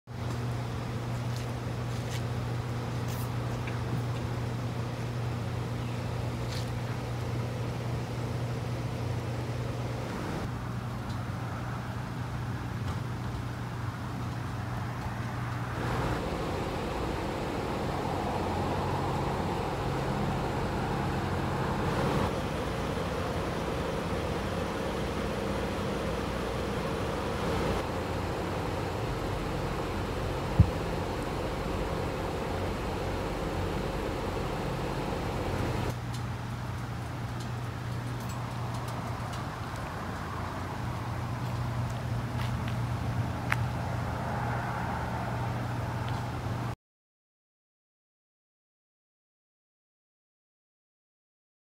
4 shot at apartment at Beach and Kernan
The Jacksonville Sheriff's Office says two women and two men were shot at the Volaris Apartments Wednesday night.